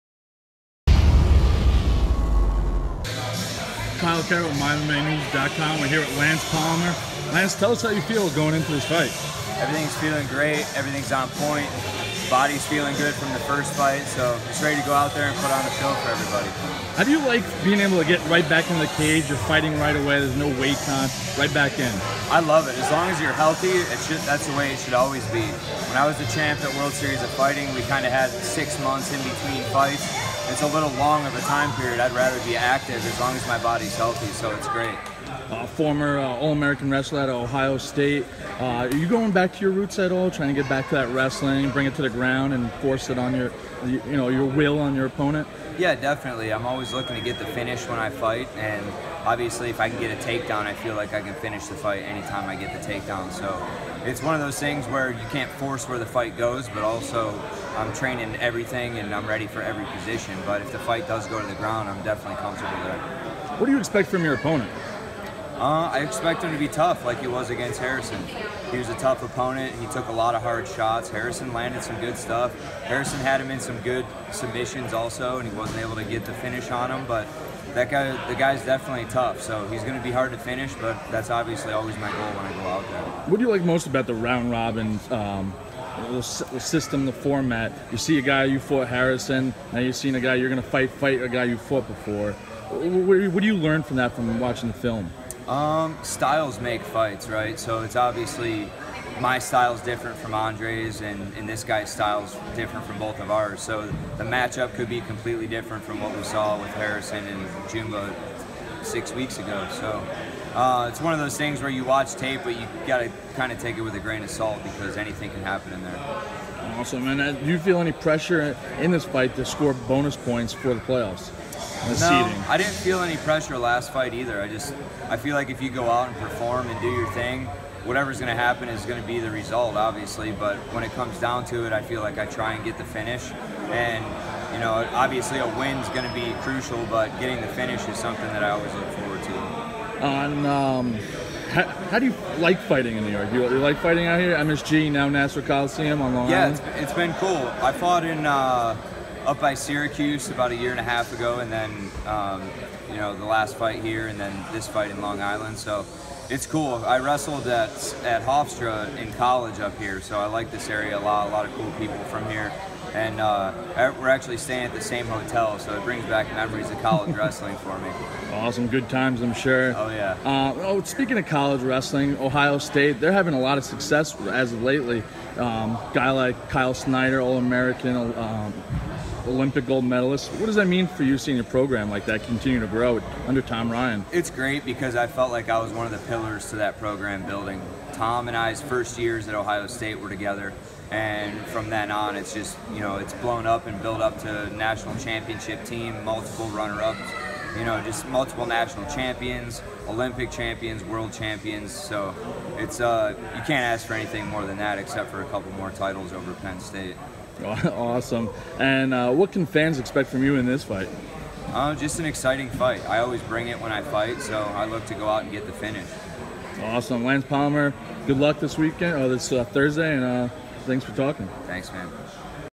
Featherweight Lance Palmer (13-3) PFL 4 Open Workout Interview
Former Ohio State ALl-AMERICAN wrestler seeks the million dollars in the PFL tournament. PFL 4 takes place Thursday, July 19, 2018.